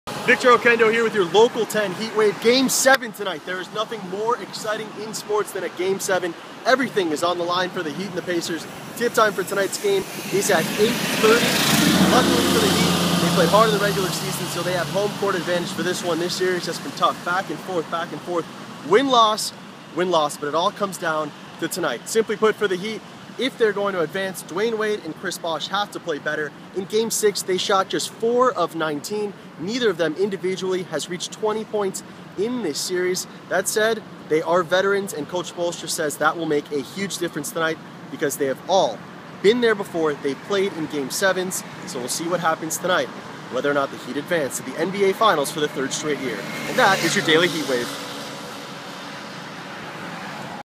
Daily Heatwave - June 3, 2013
In today's Daily Heatwave, Victor Oquendo has all the latest as the Heat prepare for the deciding Game 7 of the Eastern Conference Final on Monday vs. the Pacer.